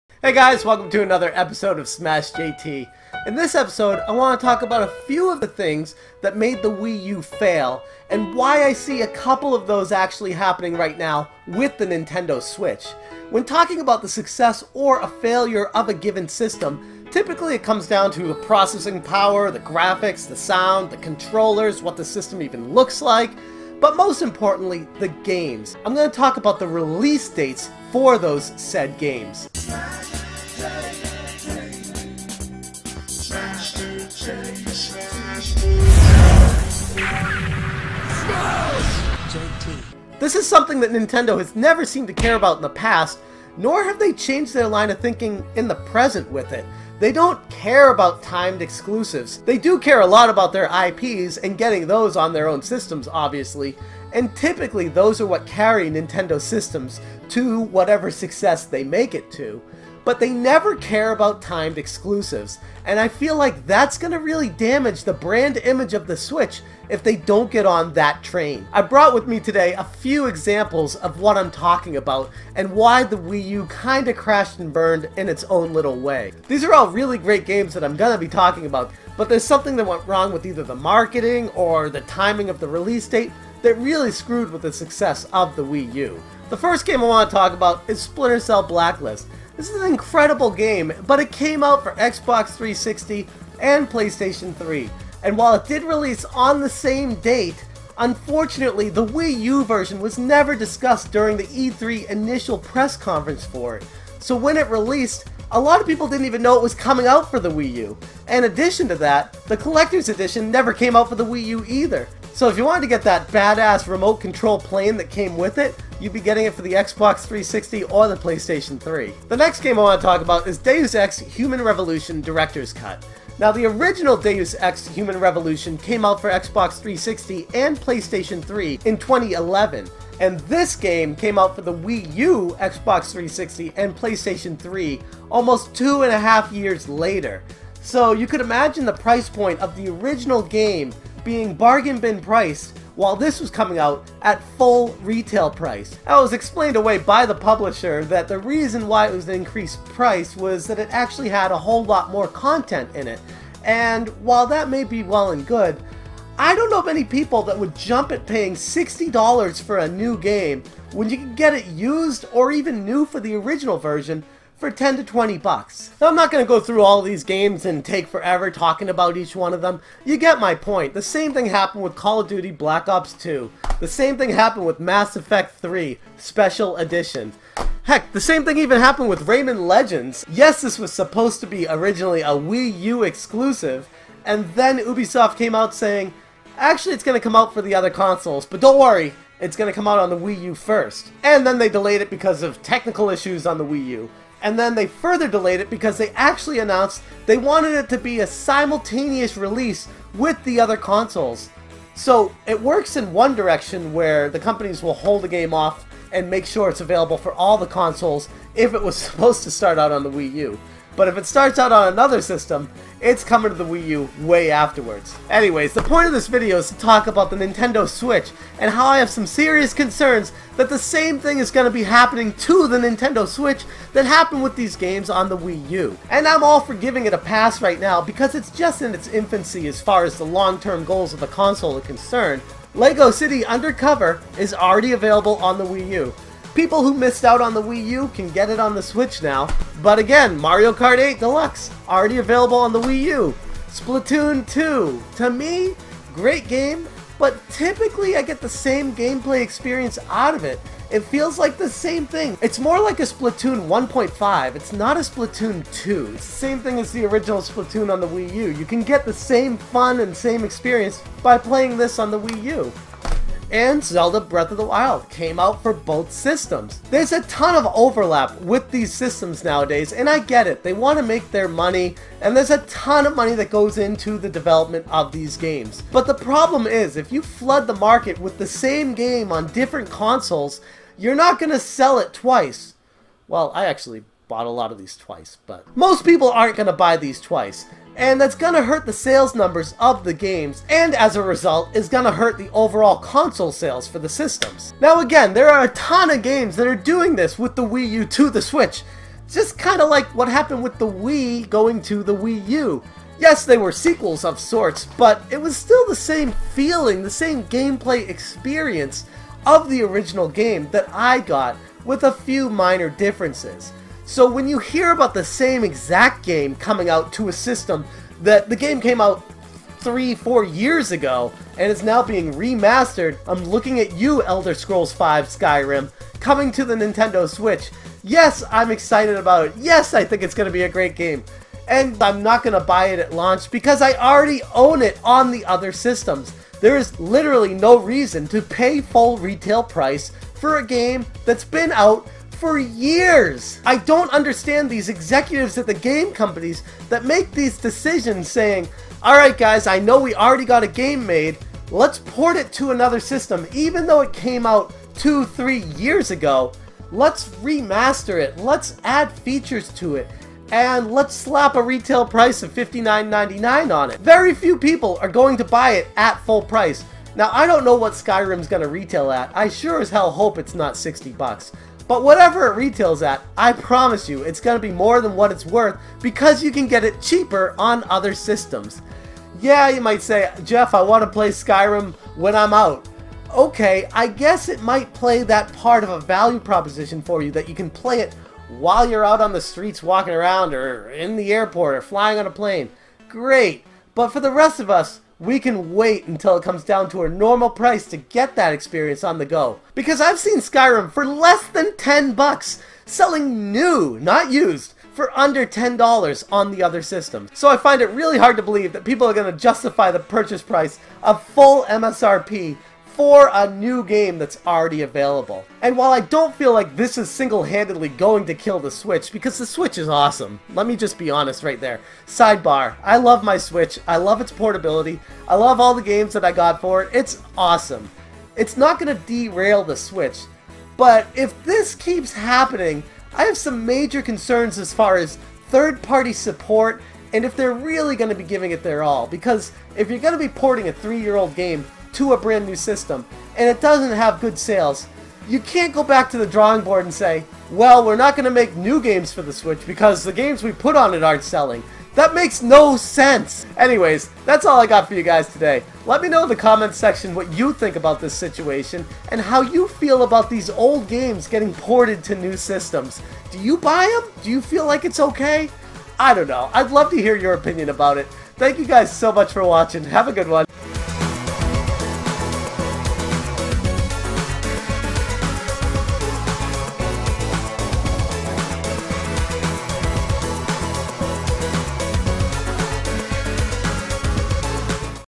Will the Nintendo Switch FAIL?
While sales of the Nintendo Switch are off to an incredible start, with some of the best games on any platform. However, there are some minor concerns that could balloon into major catastrophes if publishers don't change course and smarten up.
What are your thoughts on the future of the Nintendo Switch?